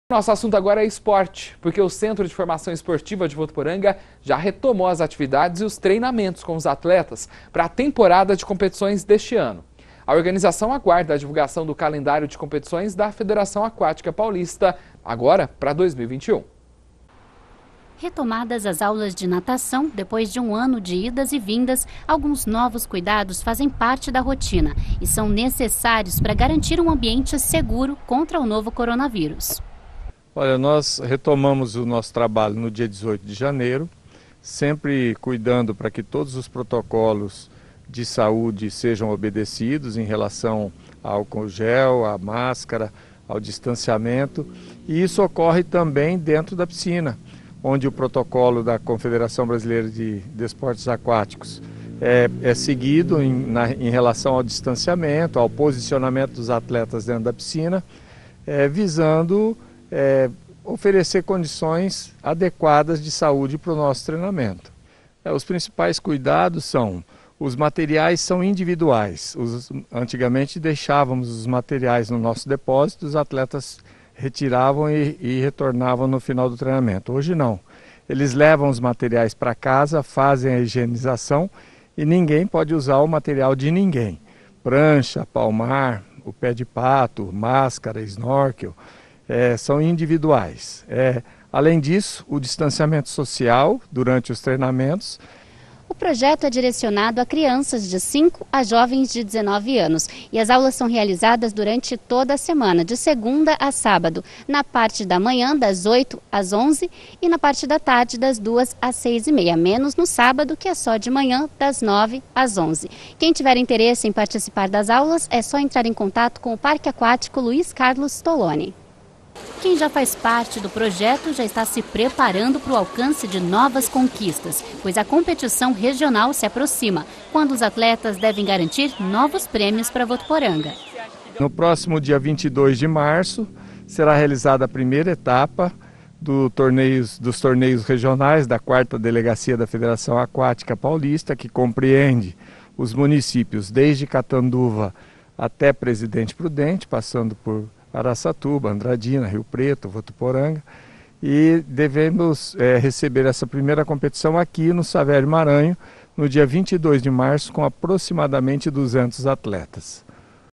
Retomada da natação
O Centro de Formação Esportiva de Votuporanga, já retomou as atividades e os treinamentos com os atletas para a temporada de competições deste ano.